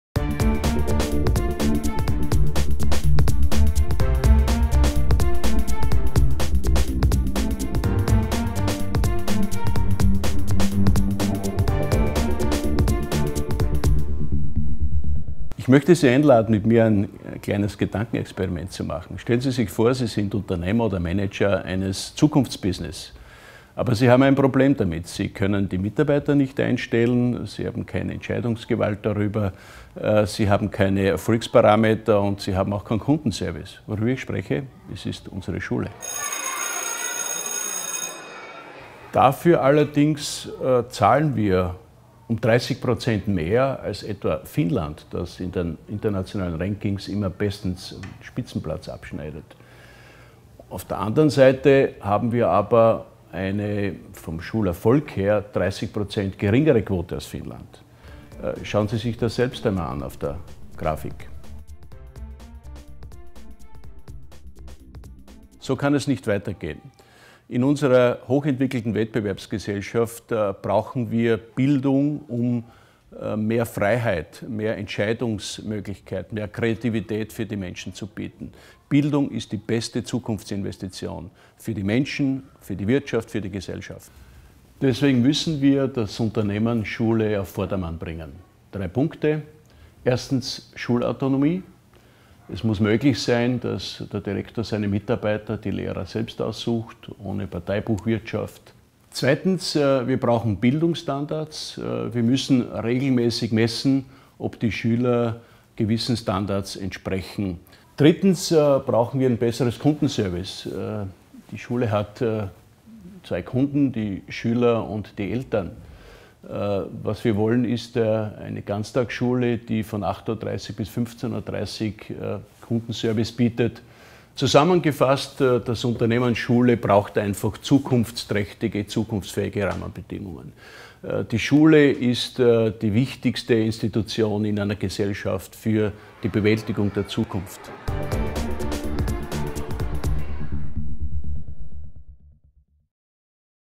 PAIERL ON POLITICS: Bildung: Unternehmen Schule
"Bildung ist die beste Zukunftsinvestition - für die Menschen und die Wirtschaft. Deswegen müssen wir das Unternehmen Schule auf Vordermann bringen", weiß mc-Präsident Paierl.